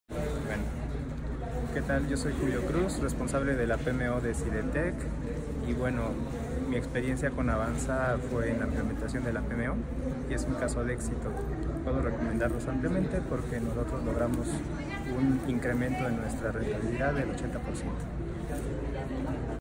Testimonio de Julio Cruz, líder de la PMO de CIDETEQ, Centro de investigación.
Tuvimos la oportunidad de colaborar con este importante centro de investigación para la implementación de su Oficina de Proyectos. Fue un proyecto muy exitoso, donde logramos integrar las buenas prácticas de los proyectos de investigación con los lineamientos del PMI. Se han logrado importantes ahorros y mejoras en los resultados finales obtenidos.  Gracias por su confianza